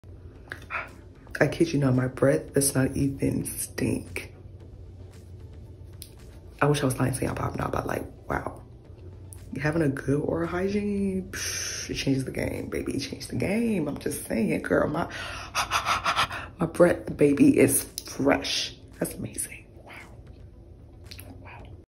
Morning after my in depth oral hygiene routine! Freeeeeesh honey!
Hi Becomers! In today’s video I’m sharing with y’all my oral routine. Since filming this I do now use a separate toothbrush for my tongue in addition to the tongue scrapper. Y’all this routine leaves my breath freeeesh honey!! What bad breath? Nada! Try it out and let me know what you think! Grab a beverage and a friend. Sit back and enjoy! God bless you and God keep you. Love y’all!❤️

Watch a vlog!